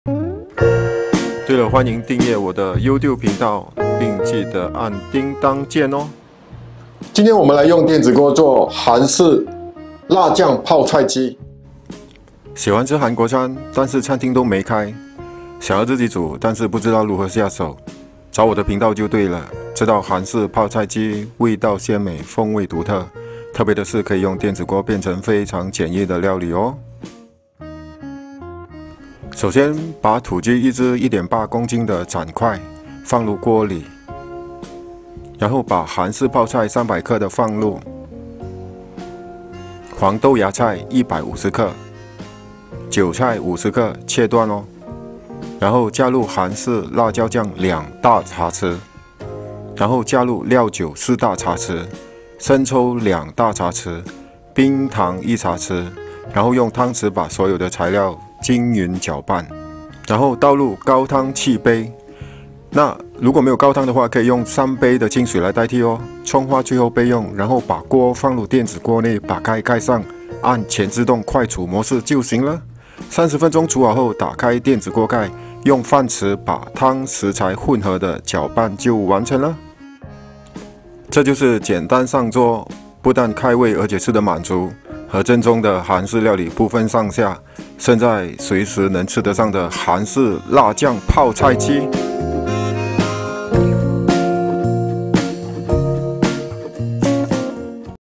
泡菜鸡 | 电子锅料理 Rice Cooker Recipe
泡菜鸡 | 电子锅料理

材料：
土鸡 1只（斩块）
韩式泡菜 300g
韭菜 50g （切段）
黄豆芽菜 150g
高汤 7杯

韩式辣椒酱 2tbs
生抽 2tbs
冰糖 1ts
料酒 4tbs
葱花 适量

Note:-
如果你的电饭锅没有“快煮模式”
就用“煮饭模式”30分钟后按“停”！